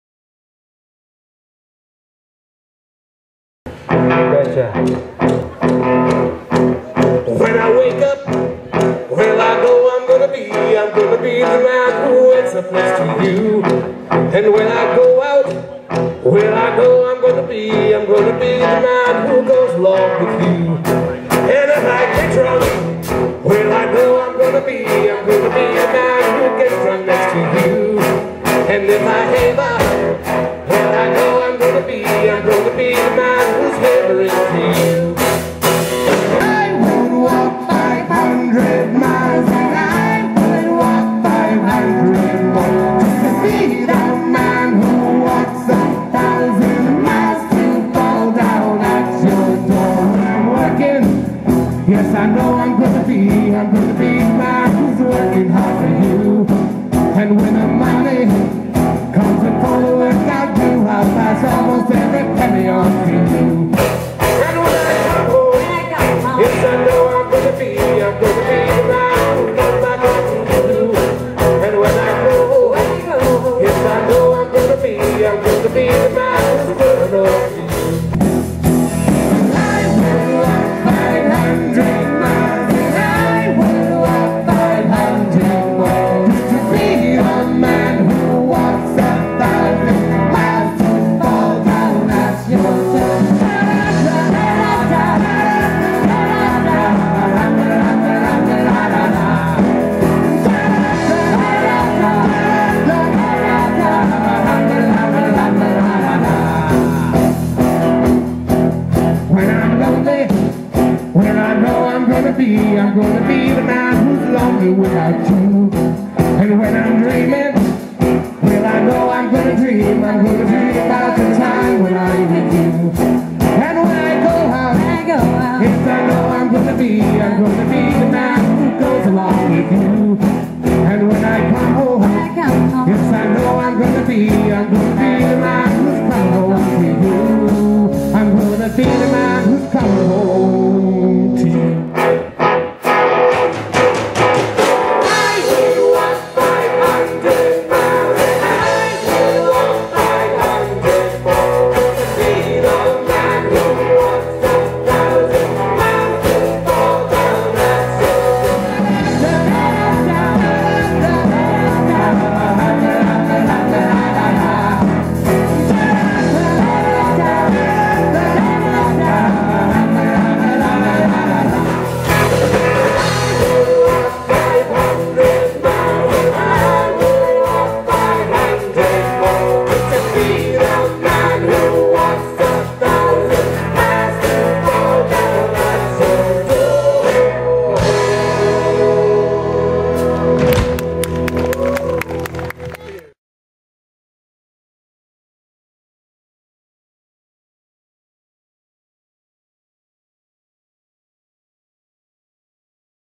Rotaryfest 2012 Coverfly at The Stage Sault Ste. Marie
Coverfly play at The Stage Sault Ste. Marie